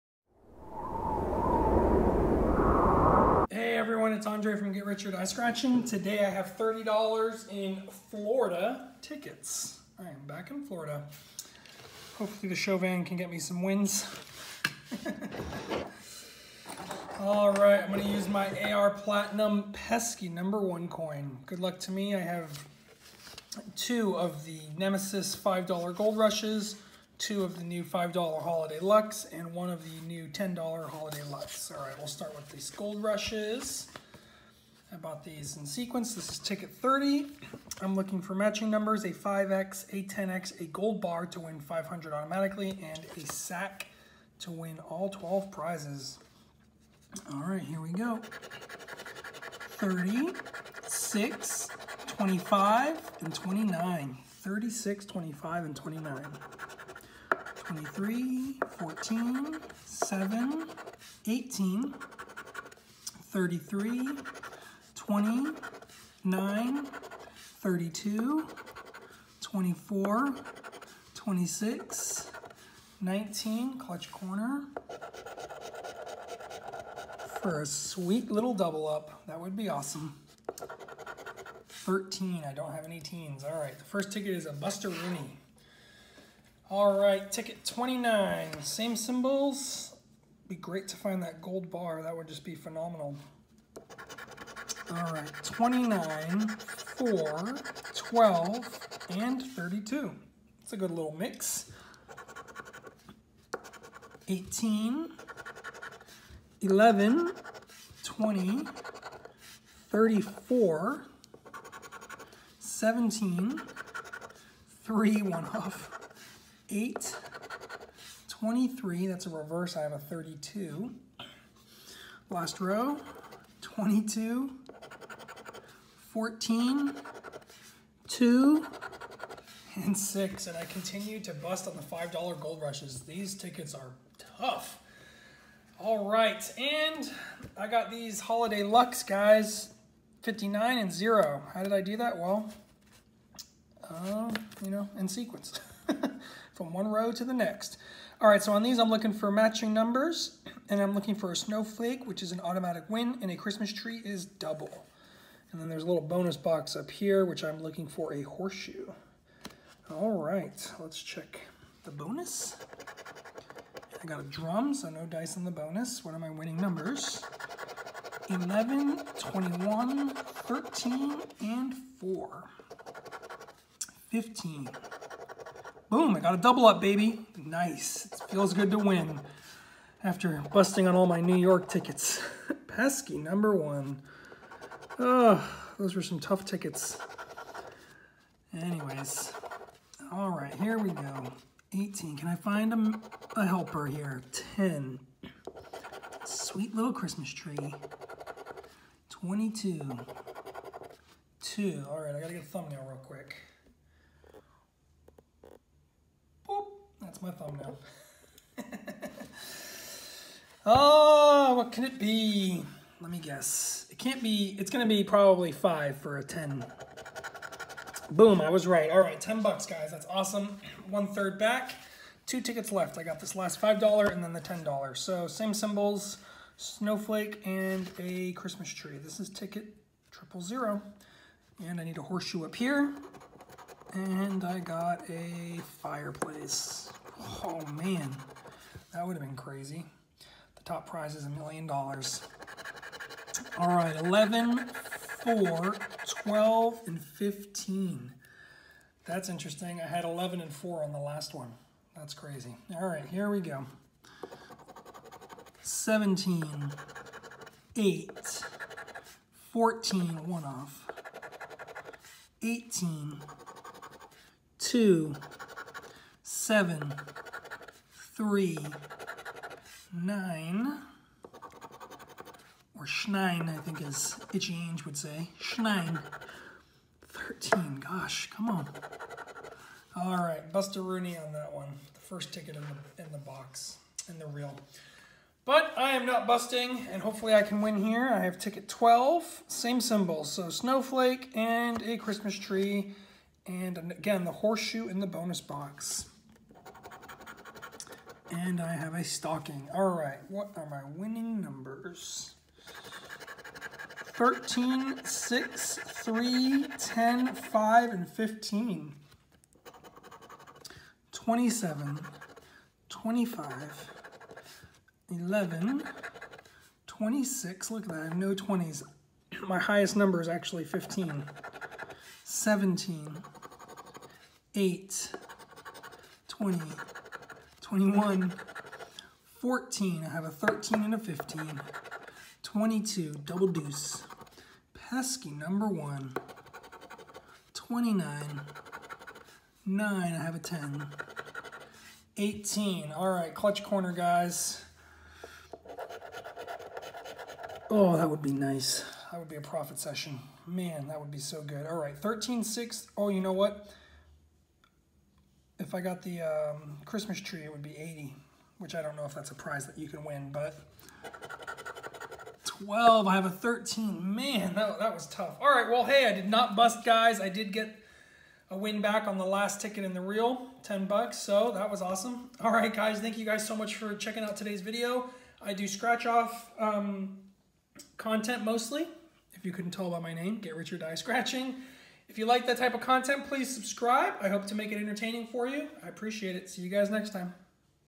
I spent $30 on Florida Lottery tickets and won!
Today I played the $5 Gold Rush, the $5 Holiday Luck and the $10 Holiday Luck and I won!